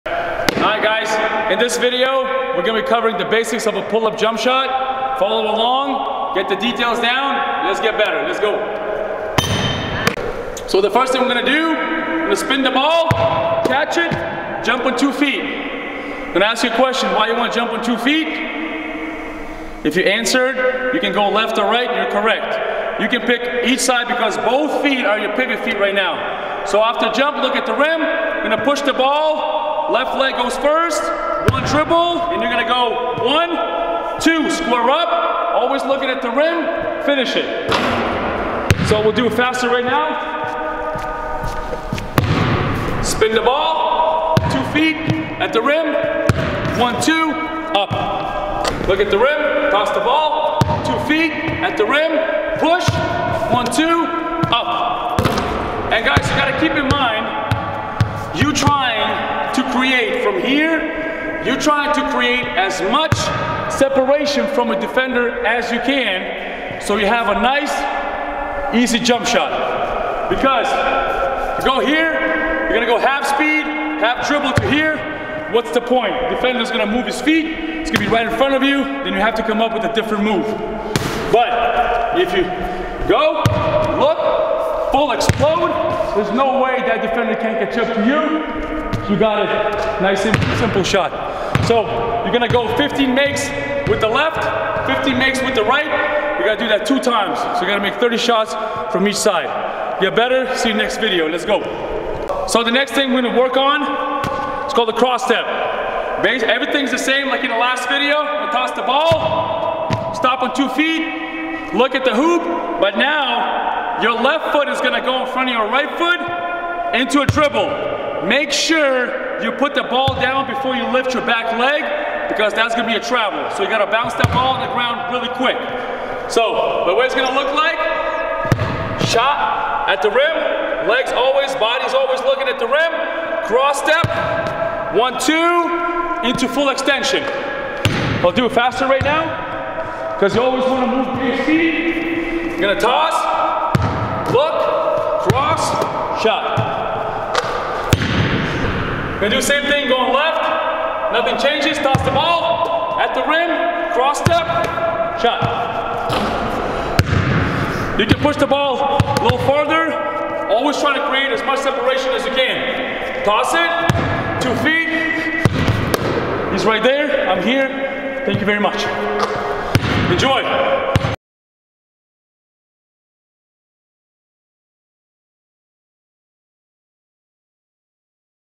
Midrange Pull Ups — Beginner Shooting 2
The Orangeville Prep Skills Academy
Beginner midrange pull ups with Evaldas Zabas:
0:00 — Intro
0:09 — Pull Up Fundamentals
1:43 — Cross Step Pull Ups